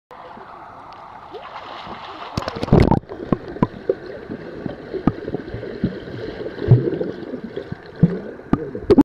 Learn to Swim Objective for a 30-month old
By 30 months of age, toddlers can learn how to swim short distances of up to 10 feet with their face in the water.  Once again, this is only possible if the prerequisite skills have been developed over the past 12-18 months.